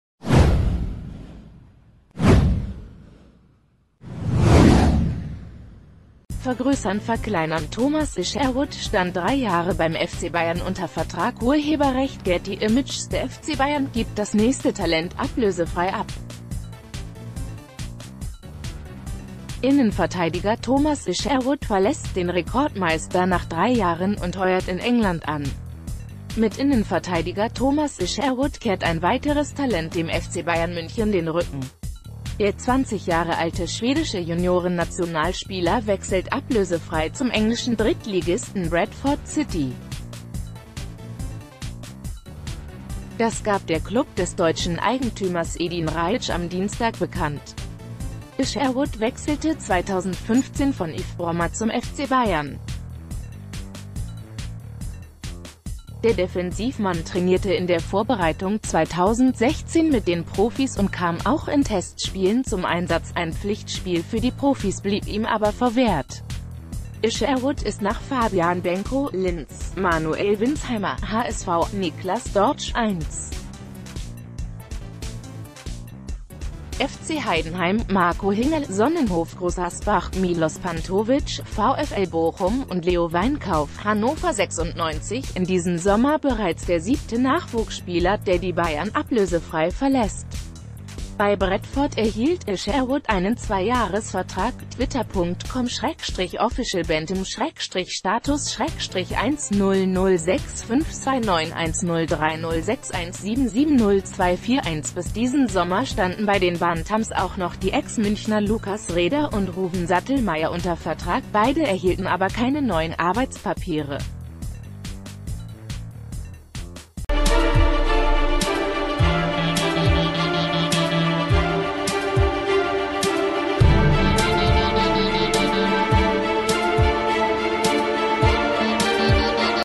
Bayern-Talent Thomas Isherwood wechselt zu Bradford City
vergrößernverkleinern Thomas Isherwood stand drei Jahre beim FC Bayern unter Vertrag © Getty Images Der FC Bayern gibt das nächste Talent ablösefrei ab. Innenverteidiger Thomas Isherwood verlässt den Rekordmeister nach drei Jahren und heuert in England an. Mit Innenverteidiger Thomas Isherwood kehrt ein weiteres Talent dem FC Bayern München den Rücken. Der 20 Jahre alte schwedische Juniorennationalspieler wechselt ablösefrei zum englischen Drittligisten Bradford City. Das gab der Klub des deutsc...